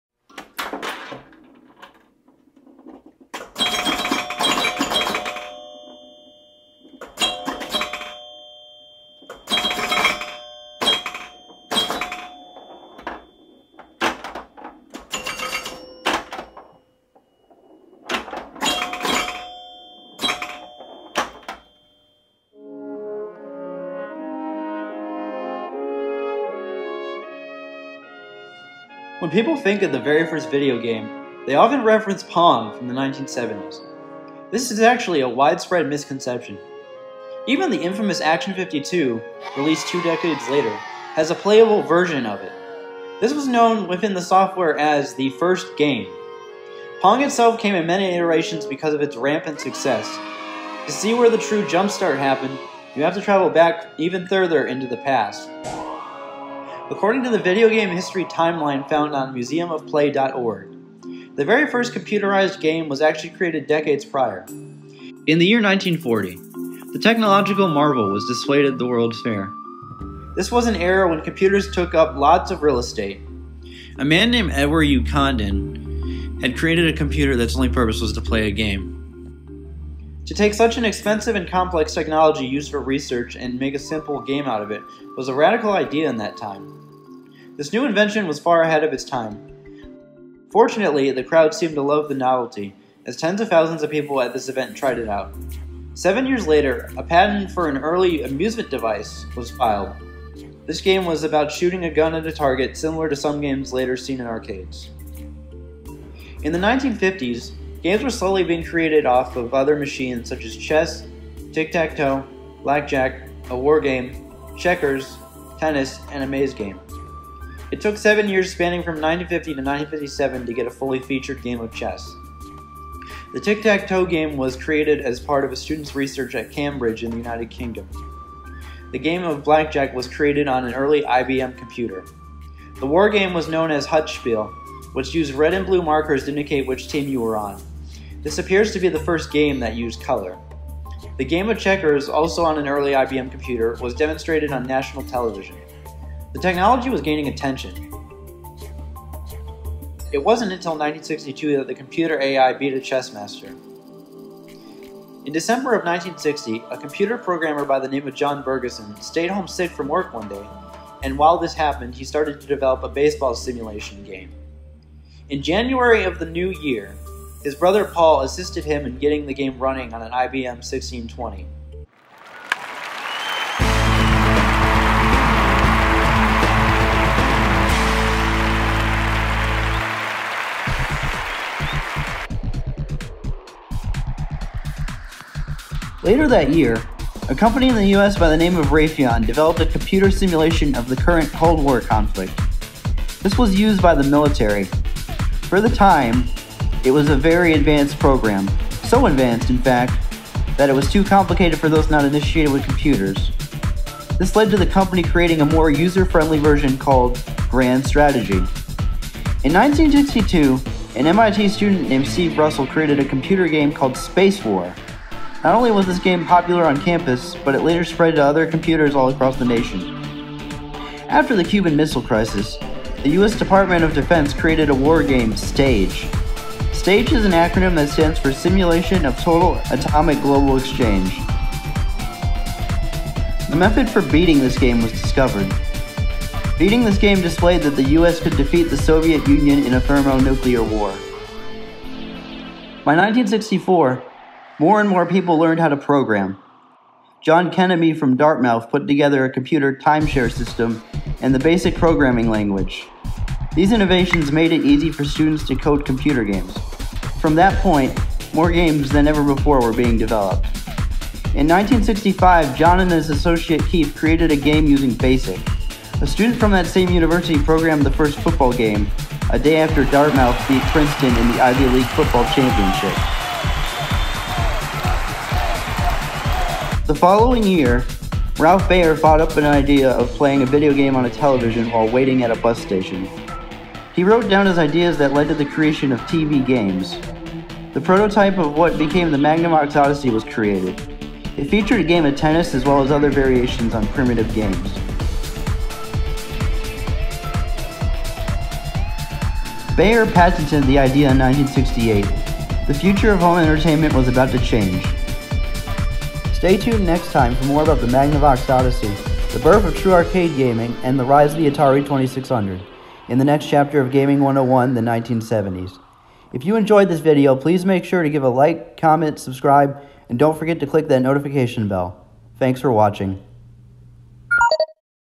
The First Few Decades of Gaming History | 1940-1970
The initial history of video games may surprise you. From early computers to devices designed specifically for the home market. A brief overview of the start of a revolution in home entertainment. This is a pilot episode in what is planned to become a series of mini documentaries. The title was changed away from Gaming History 101 post-upload because a youtube series under that name already exists.